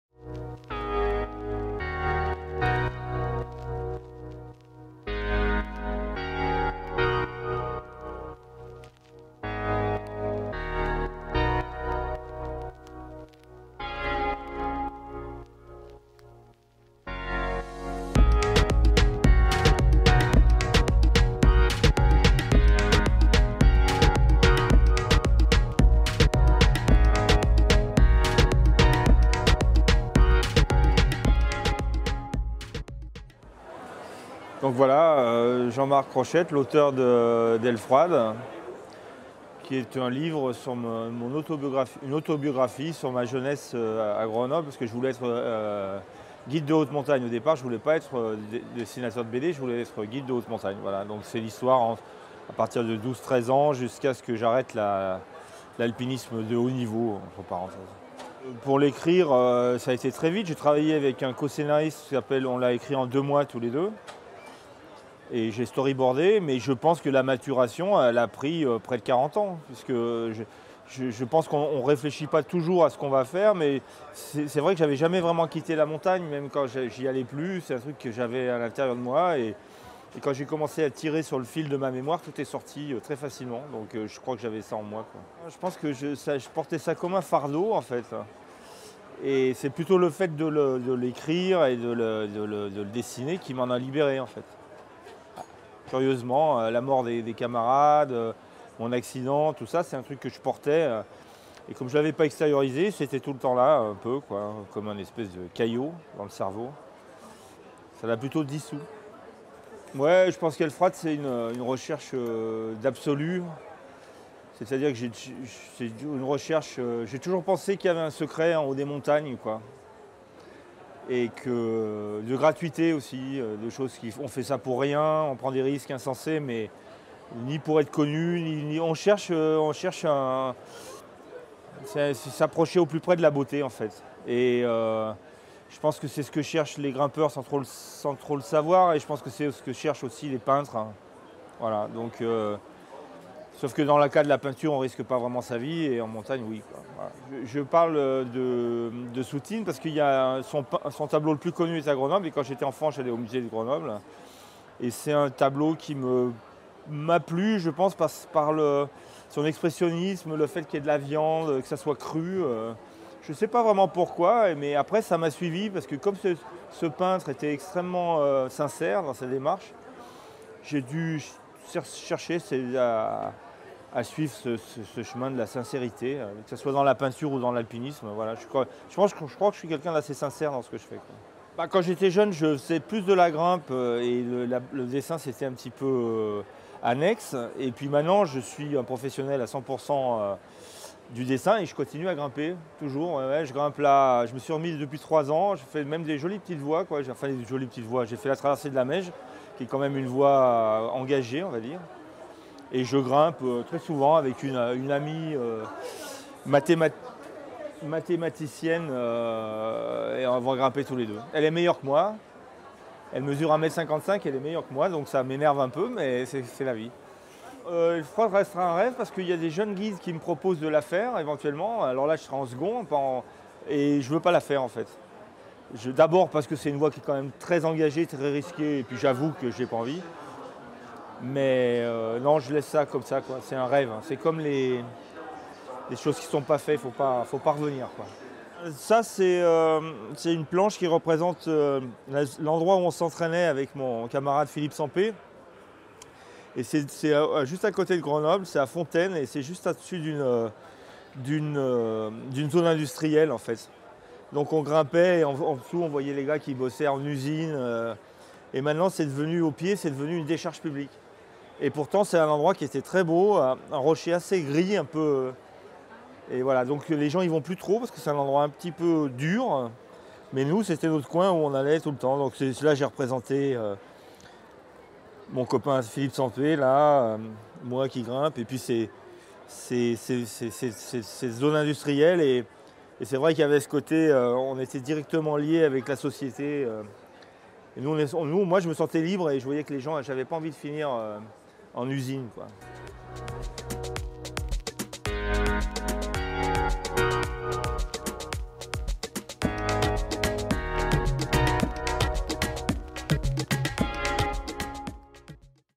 Jean-Marc Rochette - Ailefroide : altitude 3.954
À l'occasion du Festival international de la bande dessinée d'Angoulême 2019, Jean-Marc Rochette vous présente son ouvrage "Ailefroide : altitude 3.954" aux éditions Casterman.